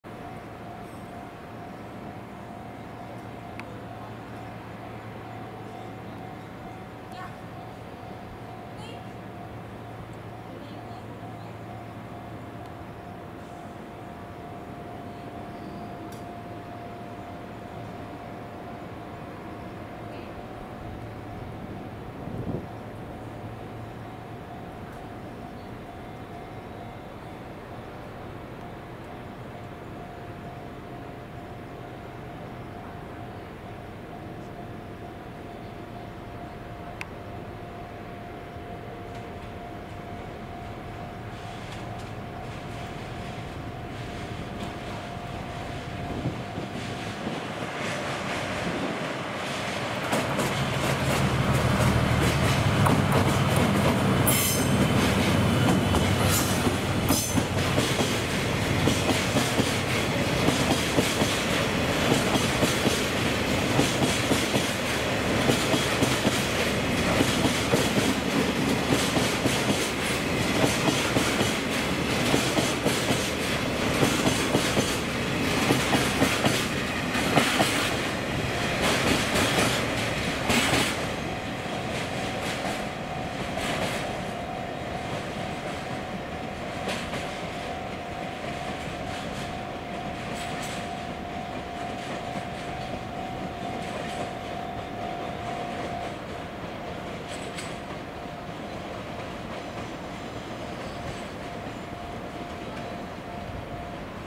【4K】関西本線 下り 貨物列車 臨8075レ DF200-201号機 [愛] 牽引 石油タンク タキ1000 JR貨物 愛知機関区 四日市 2024.9.14 S1340002
関西本線 下り 貨物列車 臨8075レ DF200-201号機 [愛] 牽引 石油タンク タキ1000 JR貨物 愛知機関区 四日市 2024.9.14 S1340002